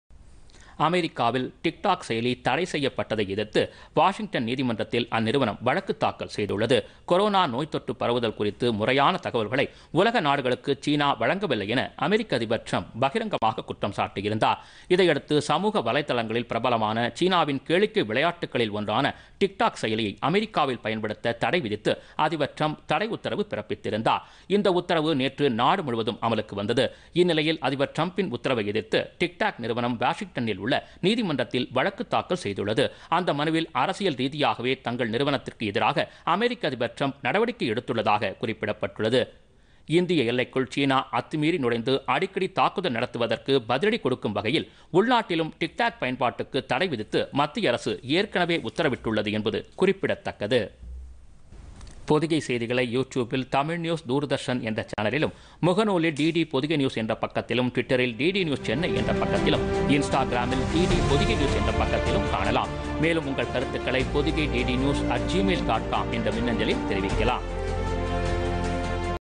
தடையை எதிர்த்து டிக்-டாக் நிறுவனம் வழக்குPodhigaiTamilNews
அமெரிக்காவில் தடை விதிக்கப்பட்டதை எதிர்த்து, டிக்-டாக் நிறுவனம் வழக்கு.Visit : TamilNews - Doordarshan